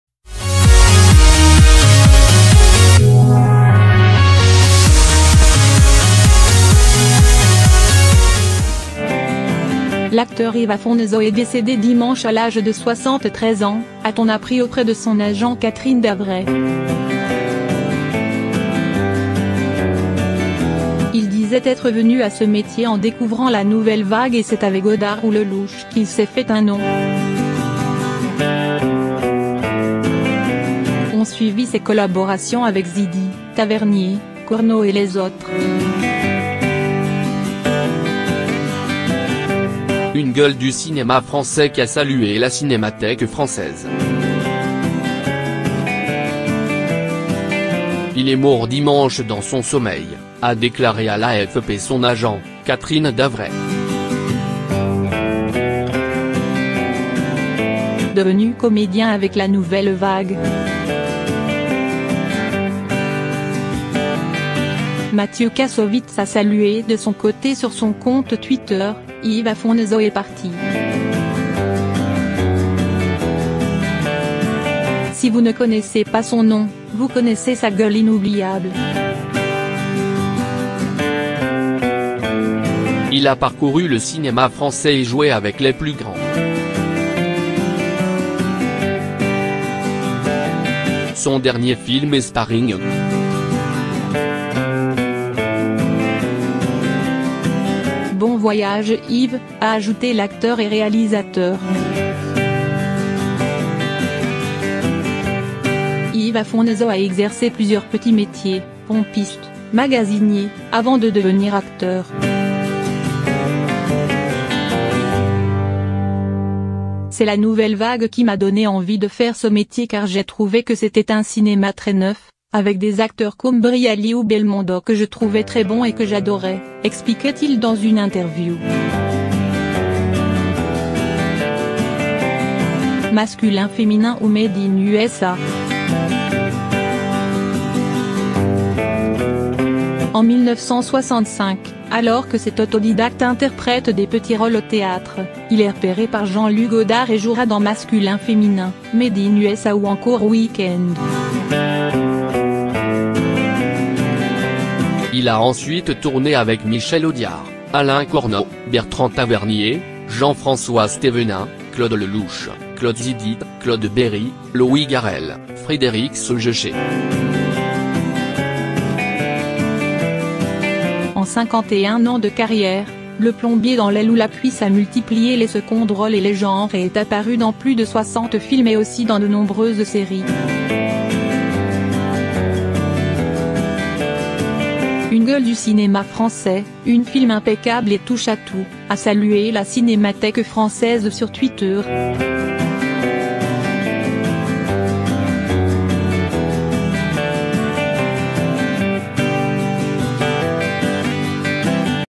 Yves Afonso: "Il est mort dimanche dans son sommeil", a déclaré à l'AFP son agent, Catherine Davray
Mort d'Yves Afonso: "Il est mort dimanche dans son sommeil", a déclaré à l'AFP son agent, Catherine Davray | LeGossipTube

L'acteur Yves Afonso est décédé dimanche à l'âge de 73 ans, a-t-on appris auprès de son agent Catherine Davray. Il disait être venu à ce métier en découvrant la Nouvelle Vague et c'est avec Godard ou Lelouch qu'il s'est fait un nom. Ont suivi ses collaborations avec Zidi, Tavernier, Corneau et les autres. Une "gueule" du cinéma français qu'a salué la Cinémathèque française…